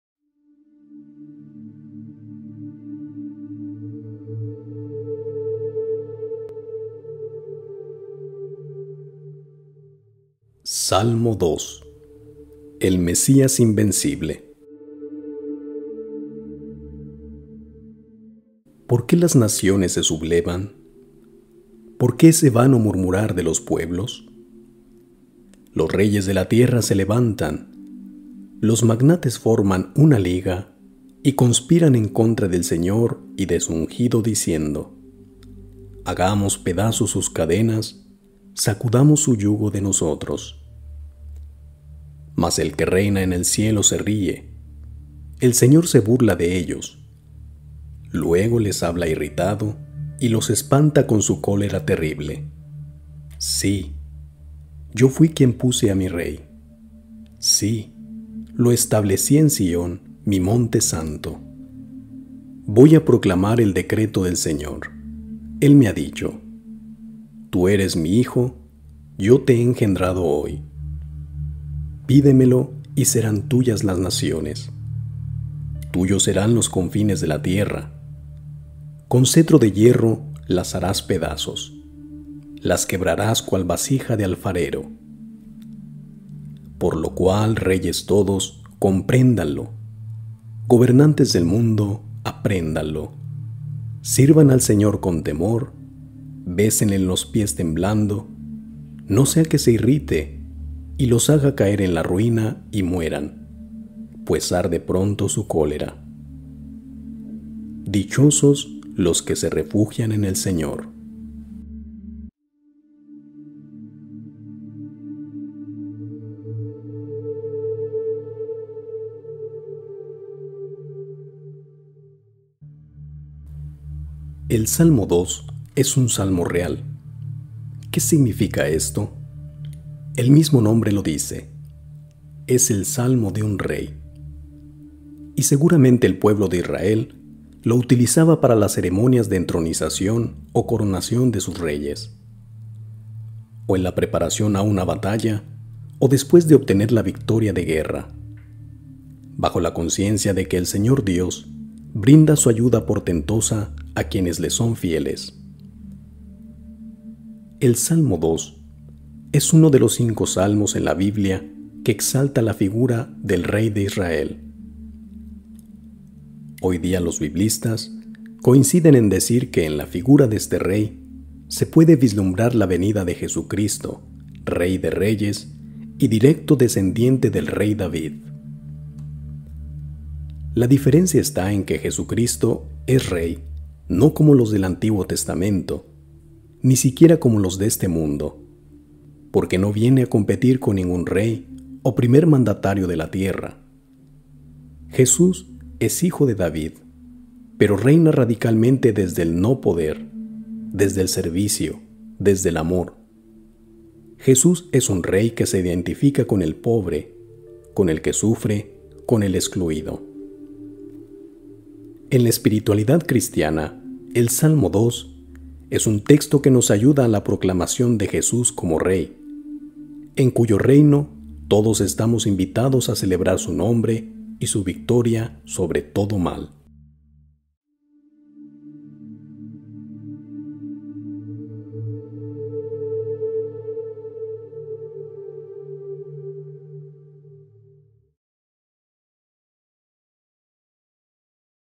Salmo 2: “El Mesías invencible”
En la espiritualidad cristiana, el Salmo 2 es un texto que nos ayuda a la proclamación de Jesús como Rey, en cuyo reino todos estamos invitados a celebrar su nombre y su victoria sobre todo mal.

Si deseas colaborar para la manutención del espacio, lo puedes hacer en:

– O deposita en la Tarjeta Banamex: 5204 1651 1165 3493 a nombre de Rafael Espino (México)